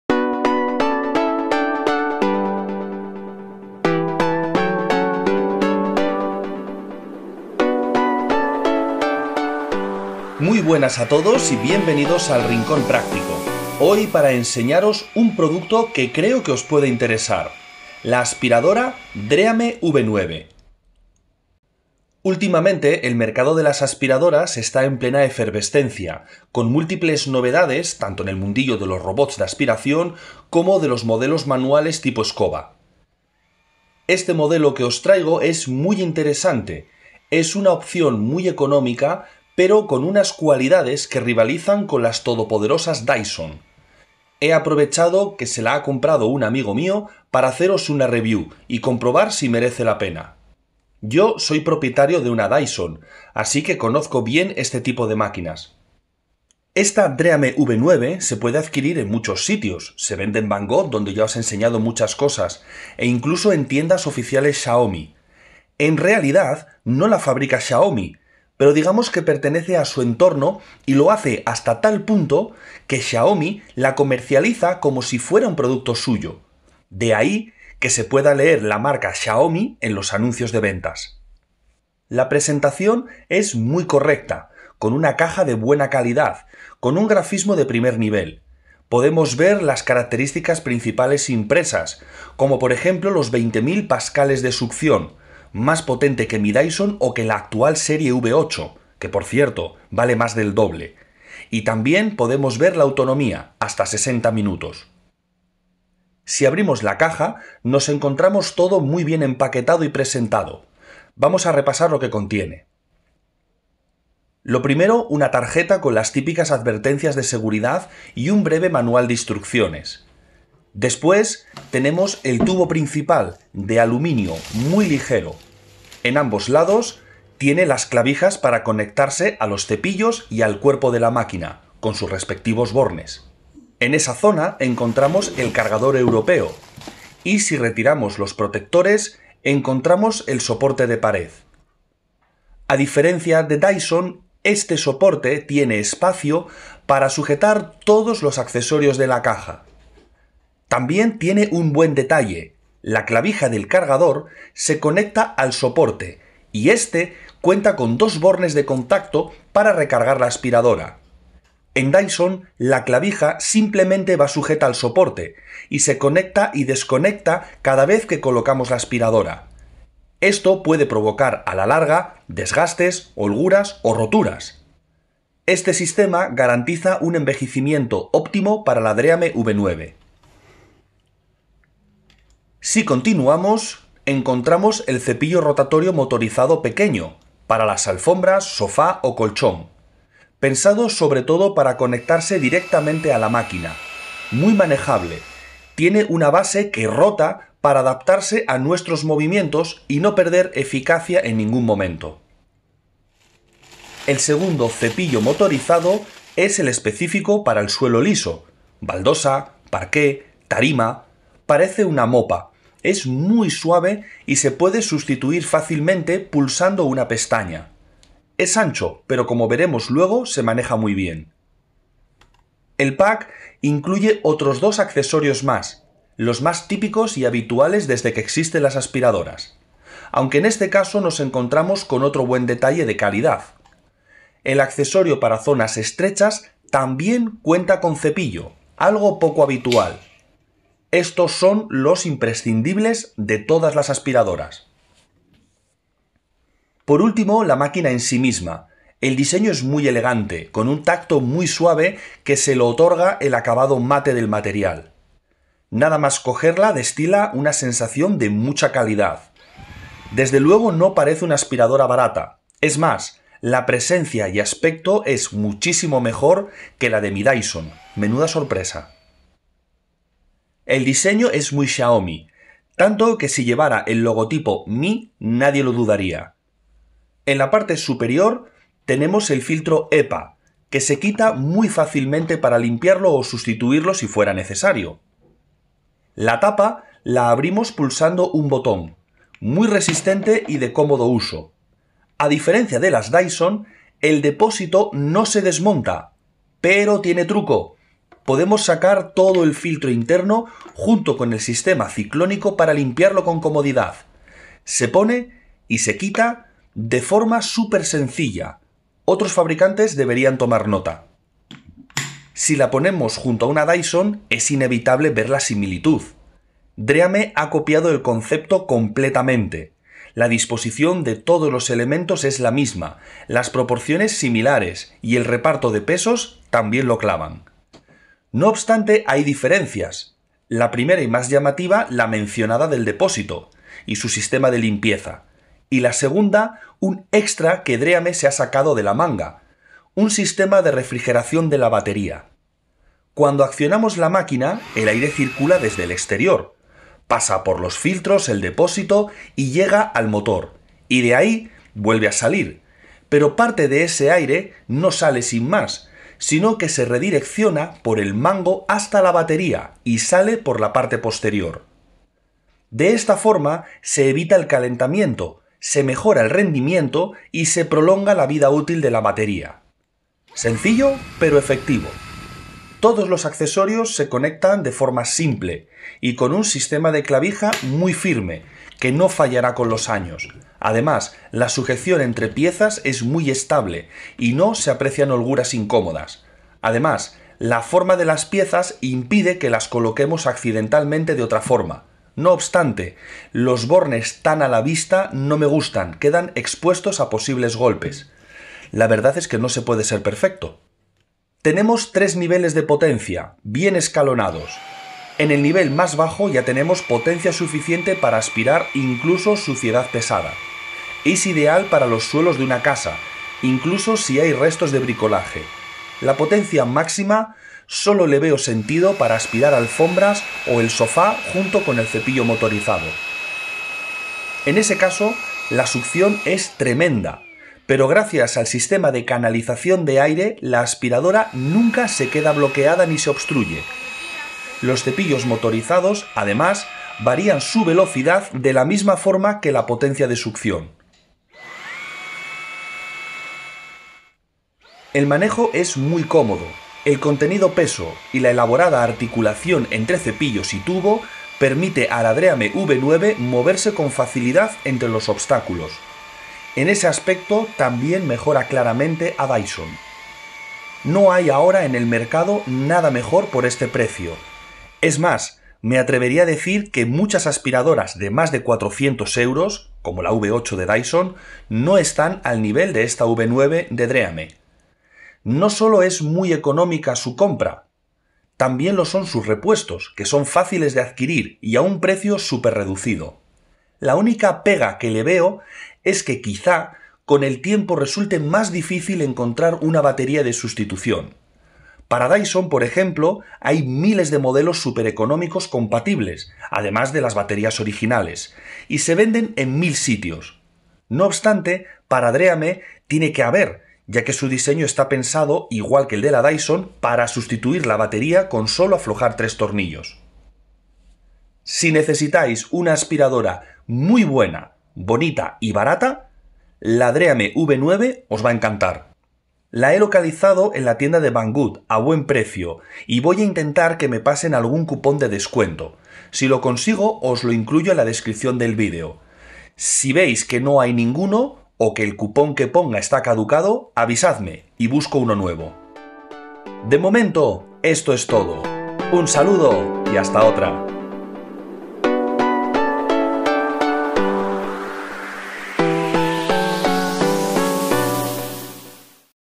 Aspirador DREAME V9. Mejor que DYSON por la mitad de precio. Prueba / Test / Review. 20.000 Pa.
Probamos la nueva DREAME V9 del universo XIAOMI. Una aspiradora ciclónica que mejora la experiencia DYSON por un coste muy inferior.

ENLACE DREAME V9:

ENLACE DREAME V10: